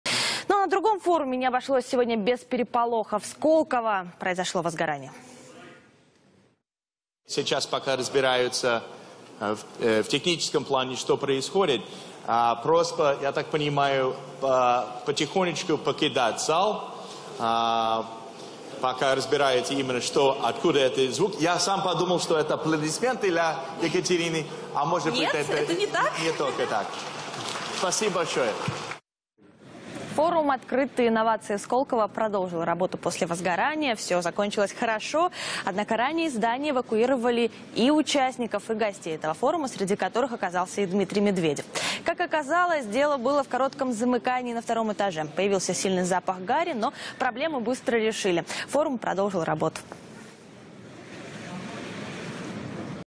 Форум в Сколково продолжил работу
Сегодня во время форума в Сколково произошло возгорание